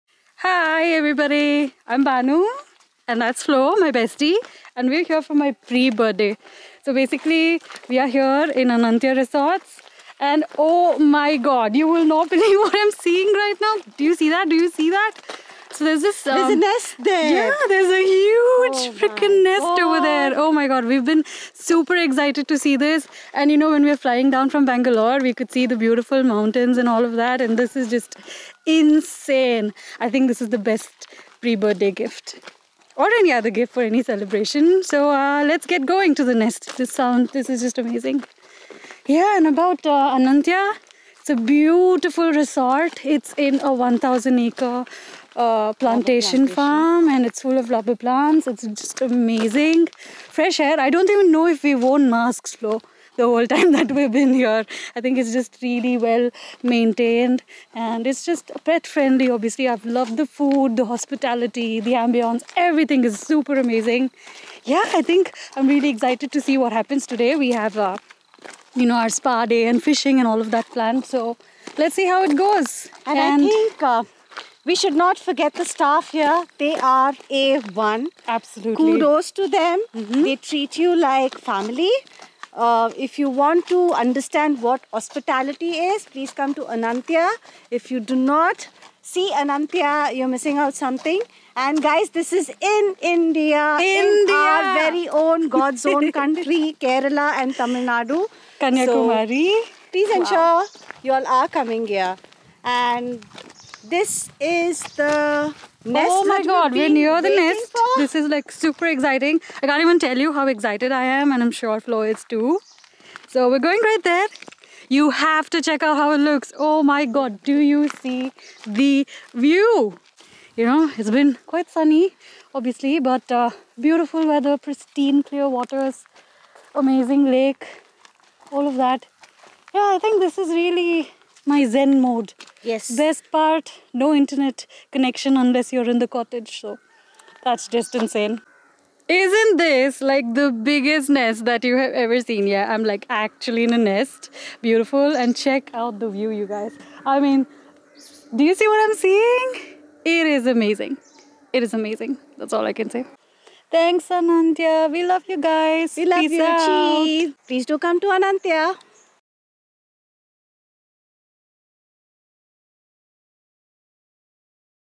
Guests from Bangalore about
Dear Florence and Banu, thank you so much for choosing Anantya for your pre-birthday celebration 🎉, and also for sparing time to share your wonderful Anantyalovenest experience with our Anantyaholics. ❤️

We appreciate your kind words for the team. We truly believe that guests like you make our resort special, giving it heart and soul and are the true inspiration behind our philosophy towards hospitality. We just can't wait to see you both again very soon. Dhanyavaadhagalu. 🙏

To book your summer vacation at Anantya dial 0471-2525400